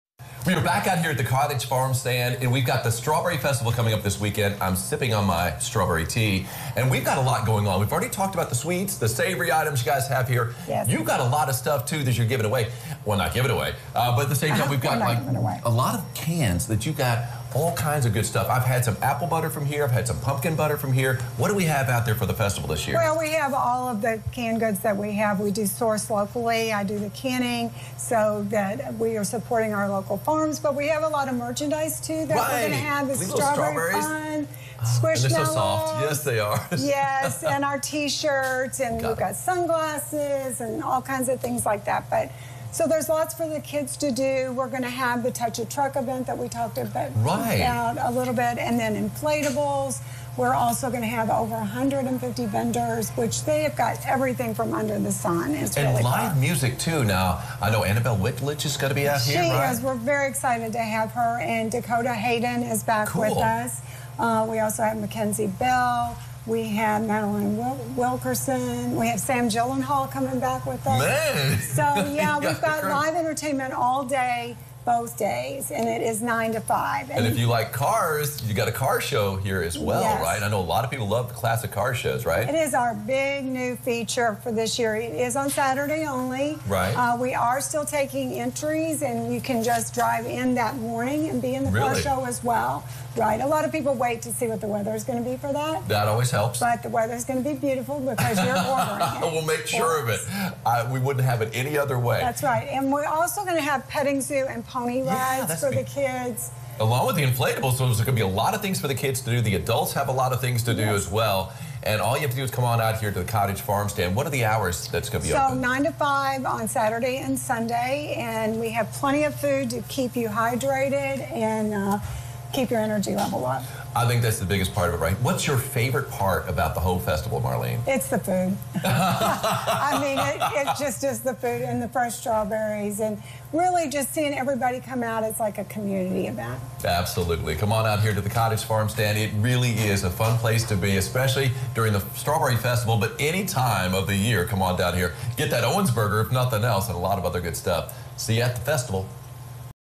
Strawberry Festival Pt 3 (6/4/24)
Ron Rhodes previews the Strawberry Festival by visiting The Cottage Farm Stand in Sorgho, KY.